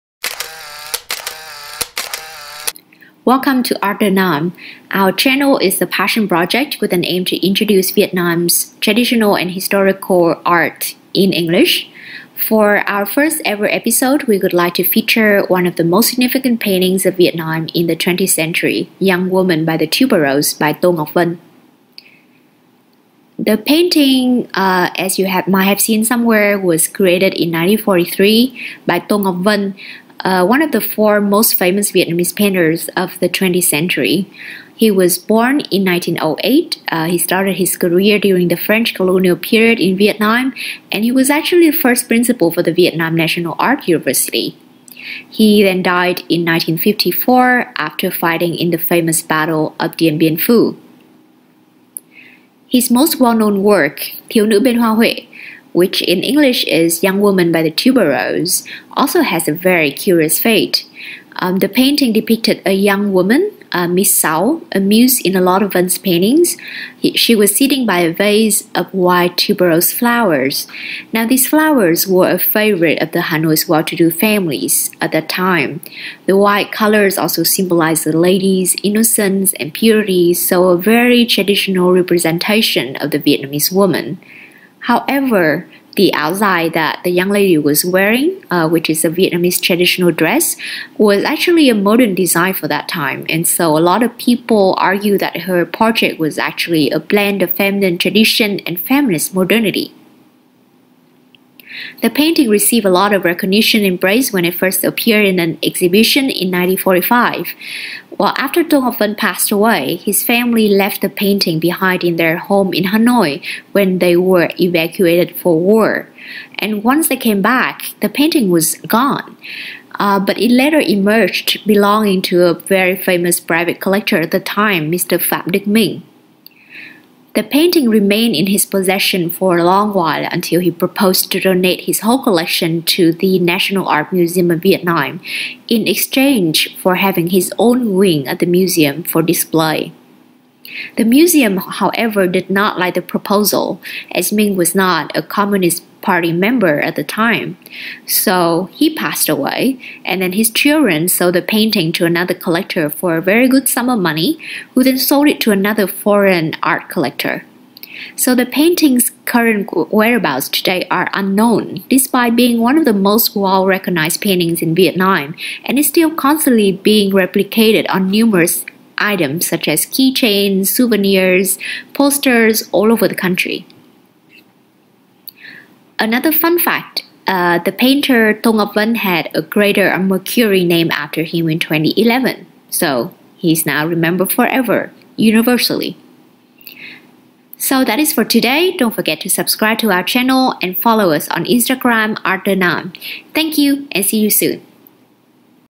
Vietnam oil on canvas - Thiếu nữ bên hoa huệ by To Ngoc Van
"Thiếu nữ bên hoa huệ"
-Young Woman by the Tuberose-

By Tô Ngọc Vân
Oil Painting, 1943
60cm X 45cm
Tô Ngọc Vân (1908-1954) was one of the four most significant Vietnamese painters of the 20th century. He started his career in French Colonial Vietnam, and was the first Principal for the Vietnam National Art University. He died in 1954 after fighting in the acclaimed battle of Dien Bien Phu. His arguably most well-known work, Thiếu Nữ Bên Hoa Huệ (Young Woman by the Tuberose), also has the most curious fate.

The painting (oil on canvas, 1943) depicted a young woman (Ms. Sau, a muse in a lot of Van’s paintings) sitting by a vase of white tuberose flowers. The tuberose flowers are a favorite of the Hanoi's well-to-do families, the white colors symbolizing a lady’s innocence and purity. However, the ao dai (Vietnamese traditional dress) the young lady was wearing was a modern design, making her portrait a blend of feminine tradition and feminist modernity.

The painting received much recognition and praise when it first appeared in an exhibition in 1945. After the painter’s death, his family left the painting behind in their home in Hanoi when they were evacuated for war. Once they came back, the painting was gone and later emerged belonging to a private collector, Mr. Pham Duc Minh. The painting remained in his possession for a long while until he proposed to donate his collection to the National Art Museum of Vietnam, in exchange for having his own wing at the museum to display his collection. The Museum, however, declined the proposal, as Minh was of the bourgeois class and not a Party member. When Minh died, his children sold the painting to another collector, who then sold it to a foreign art curator. The painting’s whereabouts today are now unknown, despite being one of the most well-recognized paintings in Vietnam and constantly replicated on numerous miscellaneous items such as key chains and posters all over the country.